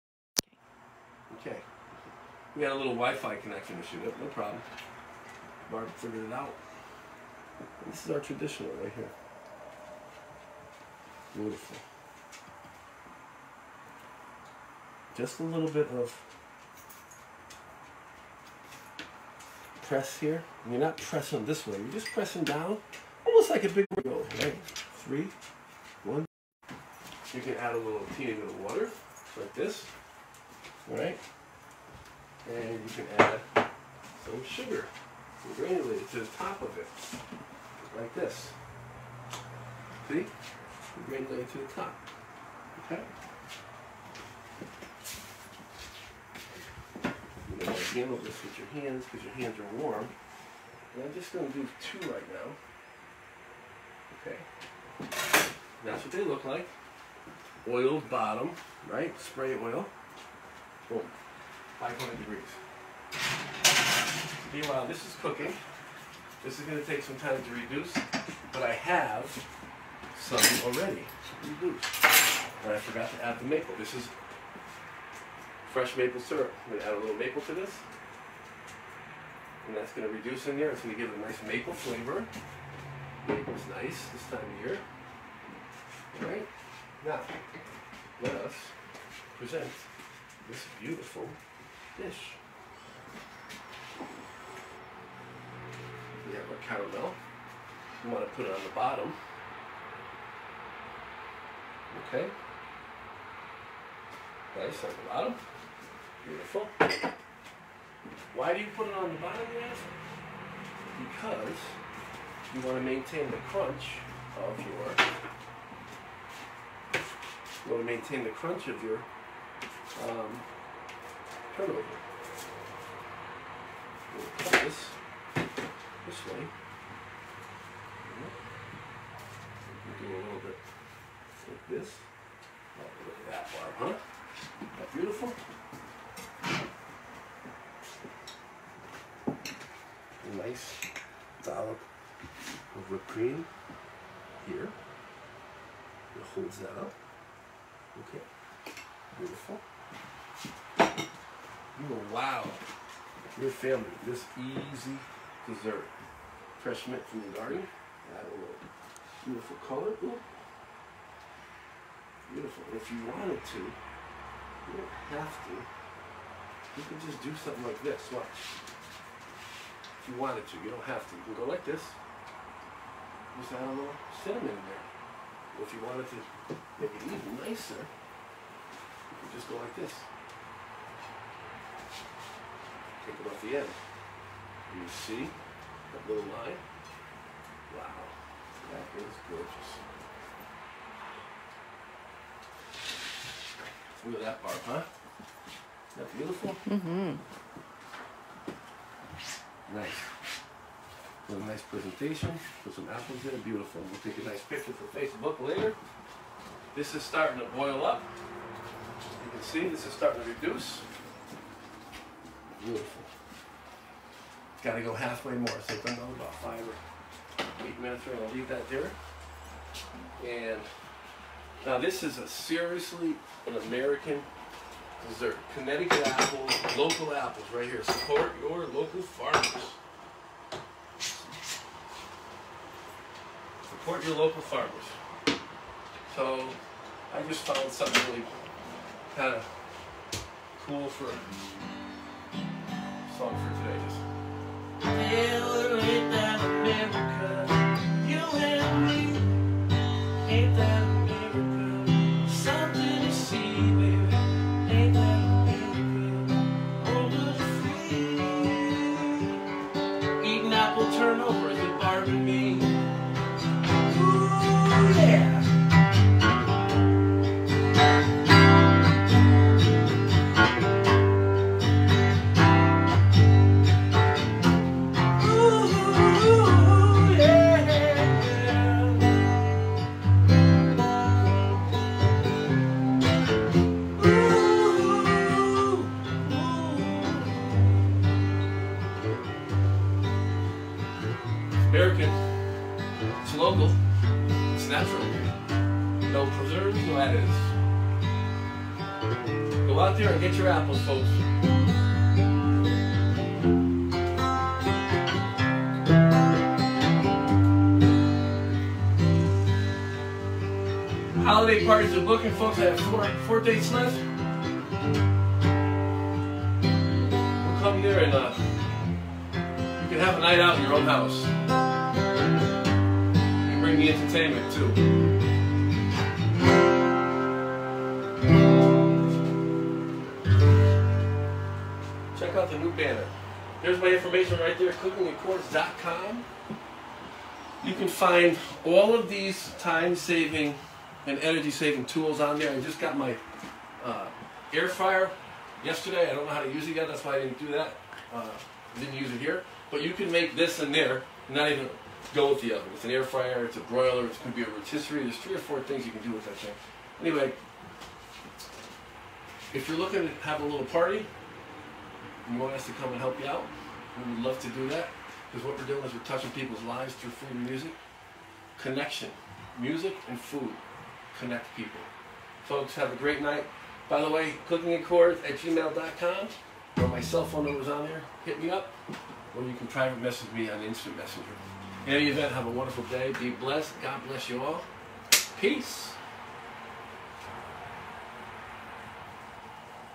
How to prepare easy Apple Turnovers Part 2
Apples are in season! Chef Danny explains how to make easy Apple Turnovers with local apples, accompanied by a maple caramel sauce.